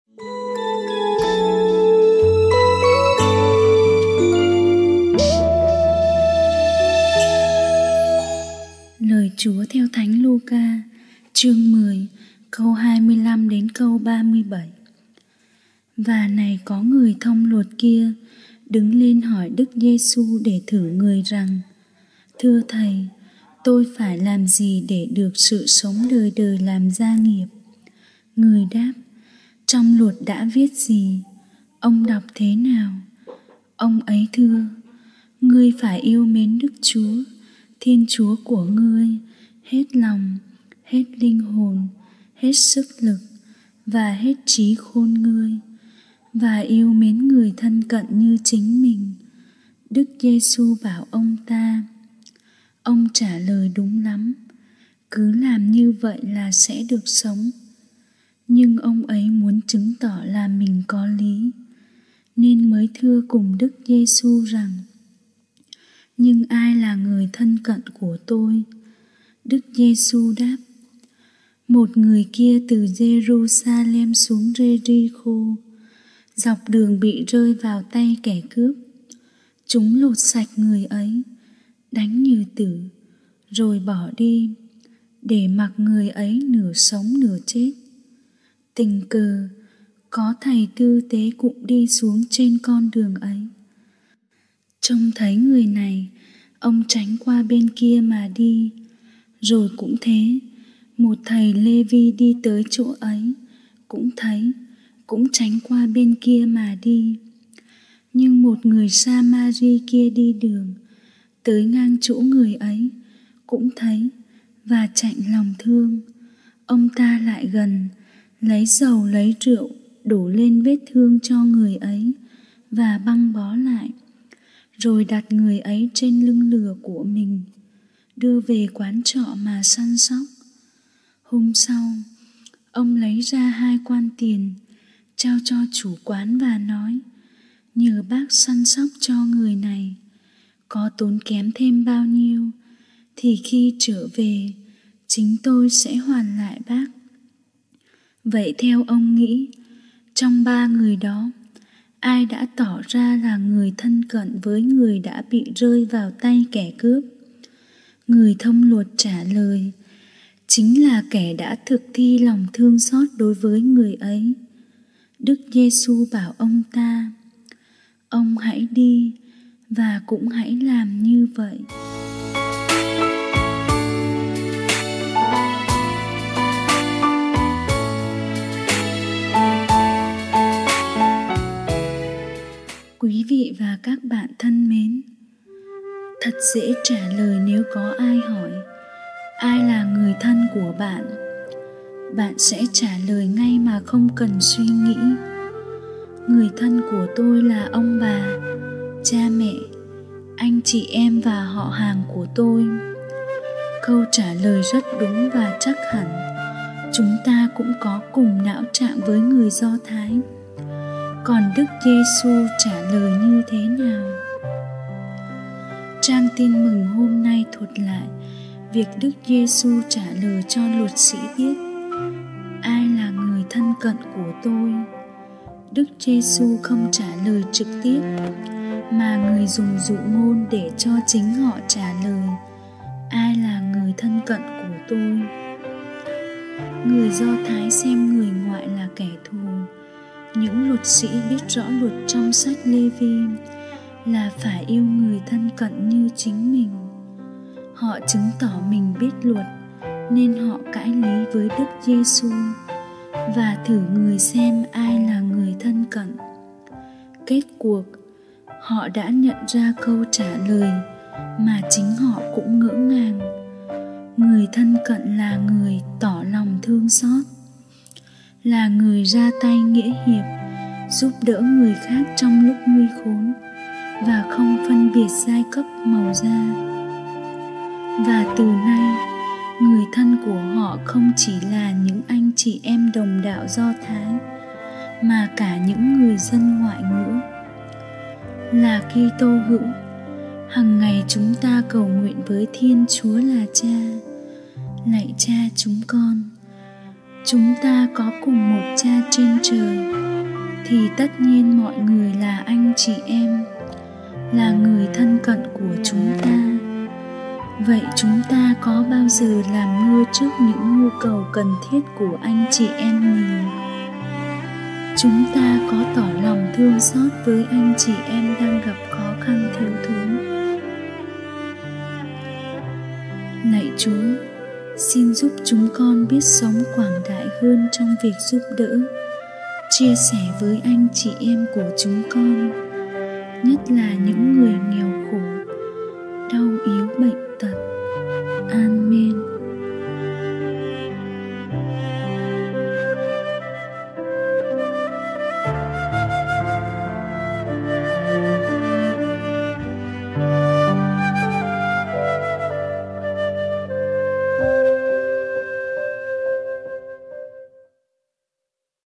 Suy Niệm Lời Chúa : Thứ Hai, 07/10/2019
thứ hai tuần XXVII thường niên C : ai là người thân cận của tôi